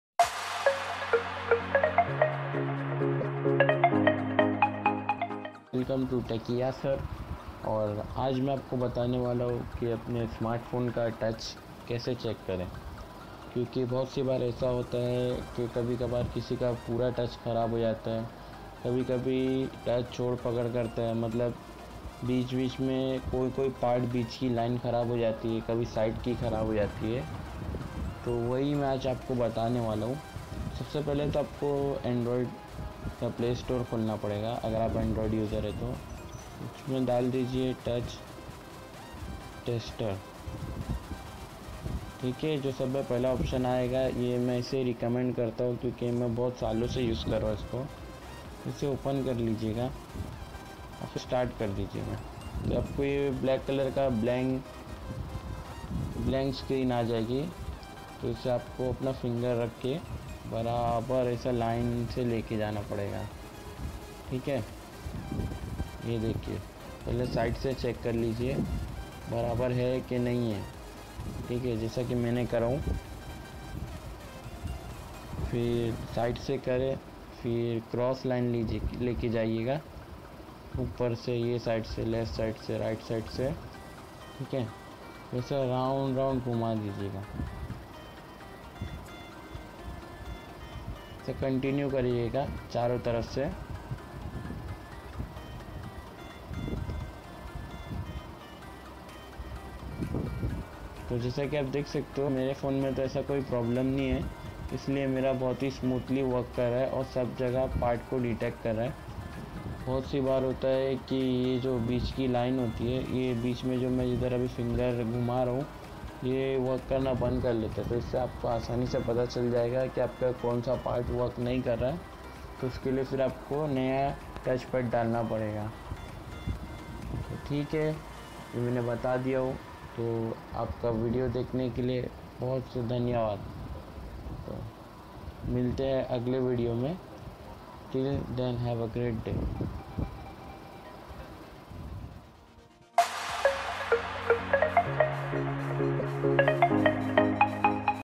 अपने SMARTPHONE का TOUCHSCREEN चेक करने का सबसे आसान तरीका!!!
अब अपने आप से करे अपने SMARTPHONE का TOUCHSCREEN चेक!!!ये सबसे आसान तरीका है!!!

EASIEST WAY TO CHECK YOUR SMARTPHONE TOUCHSCREEN!!!